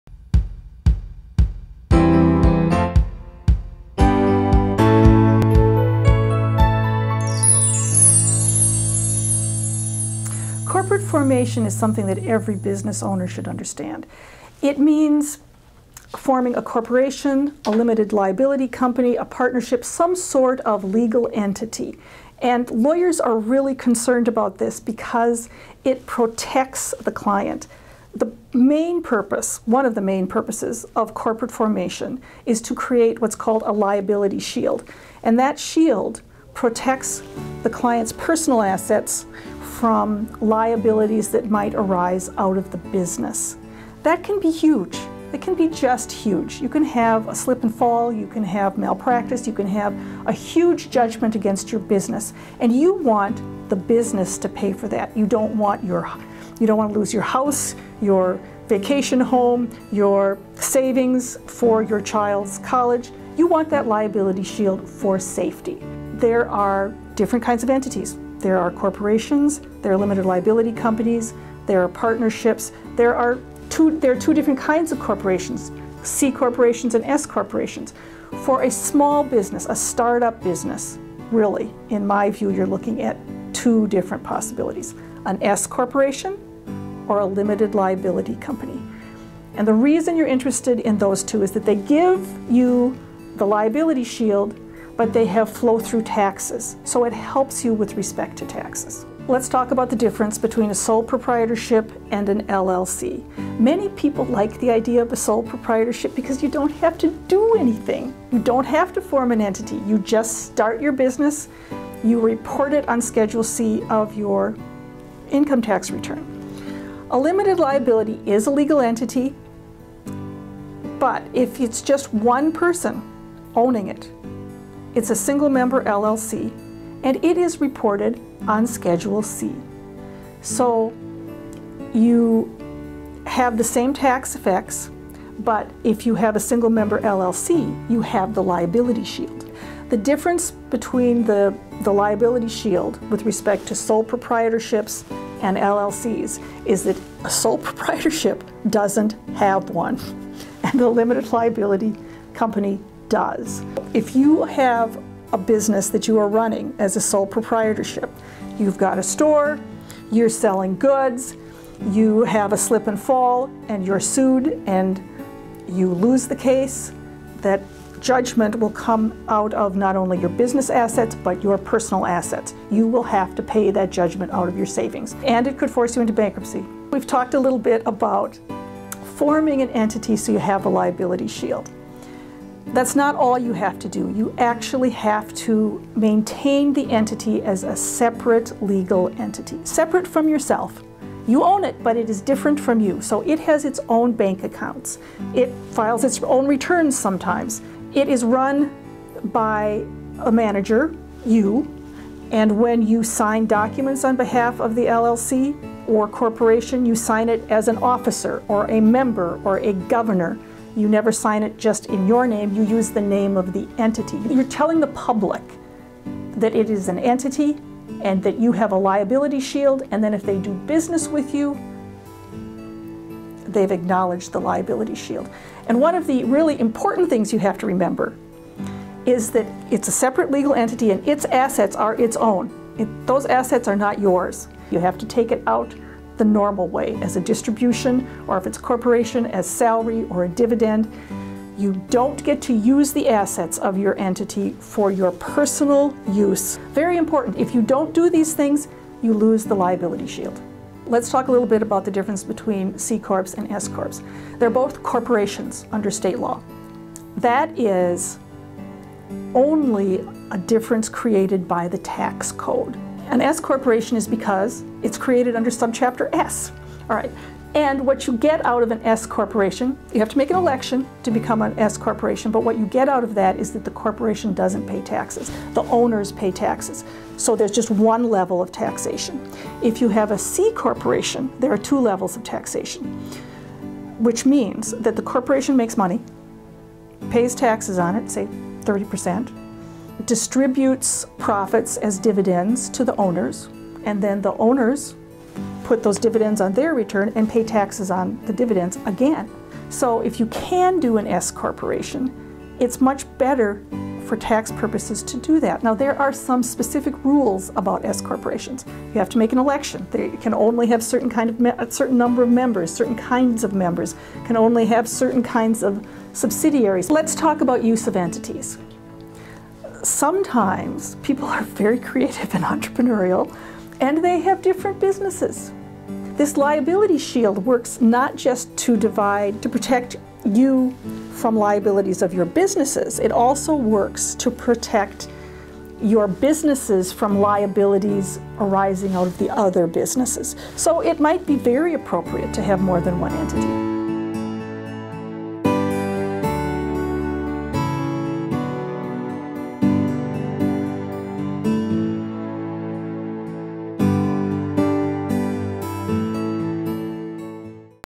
What is a Corporate Entity? Attorney 1 of 7
What is a corporate entity? Learn about the different small business corporations before talking with your attorney.

Getting a feel for the basics saves your attorney time explaining it to you, which saves you money!